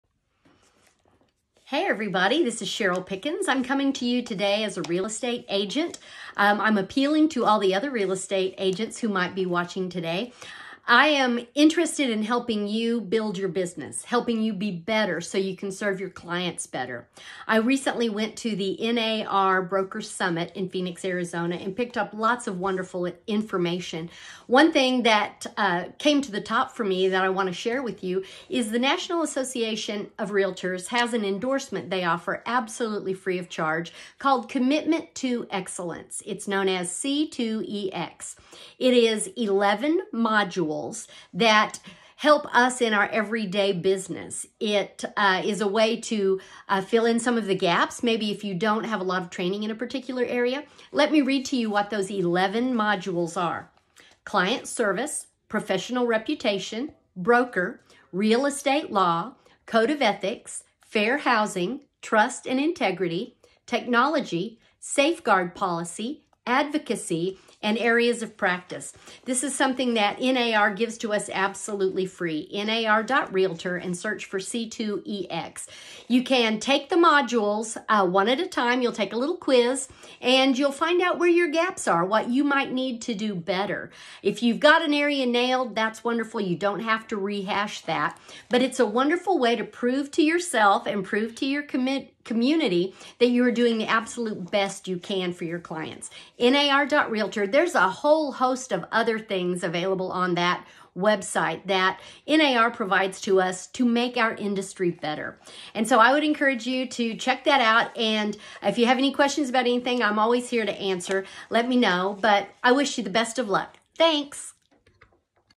C2EX Pep Talk
A reminder about the C2EX endorsement offered by NAR.